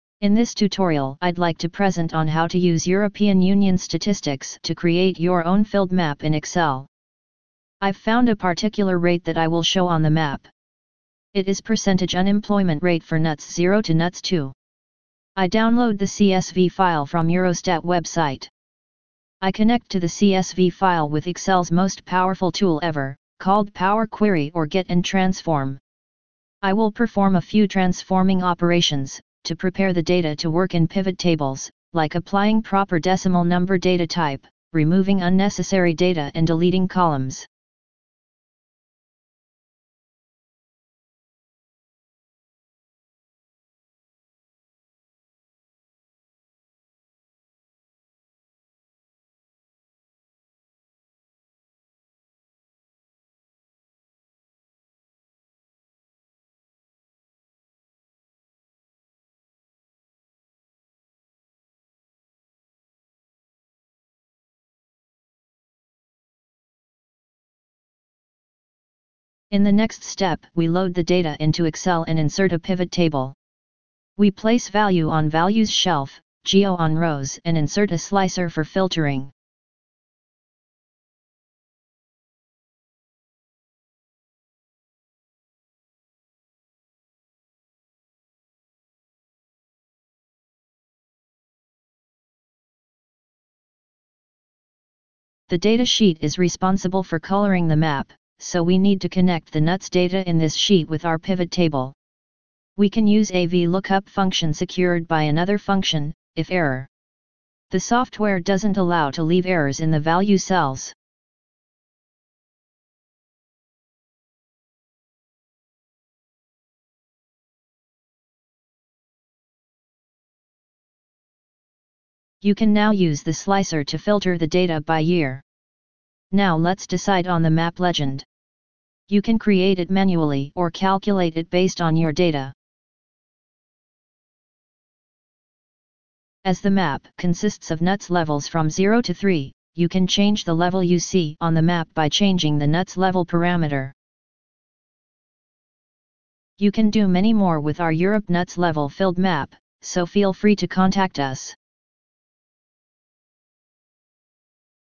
How to create a statistics map for Europe NUTS 0-1-2-3 in Excel?
In this tutorial I’d like to present on how to use European Union Statistics to create your own filled map in Excel.